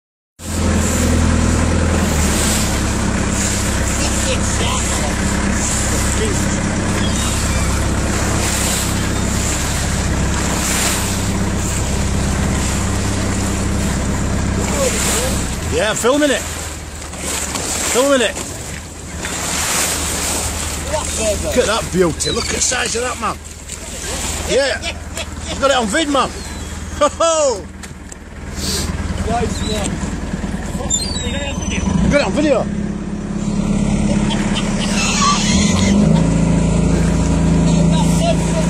Basking shark seen off the Flamborough Coastline
The basking shark was spotted and filmed by Richard Emmerson and Russ Conlon whilst out fishing on Wednesday 9th September